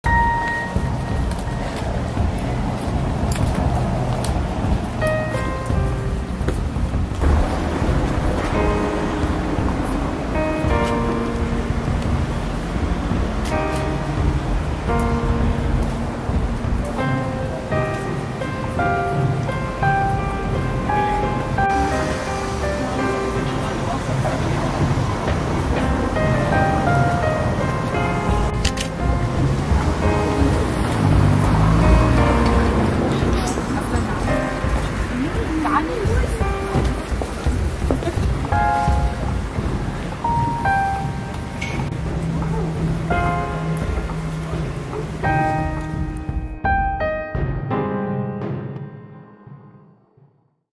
Maskenparade auf der Louisenstraße
Etwa 20 verkleidete Masken-Männer und -Frauen zogen am 3. Februar durch die Gassen der Neustadt. Eine Performance der besonderen Art.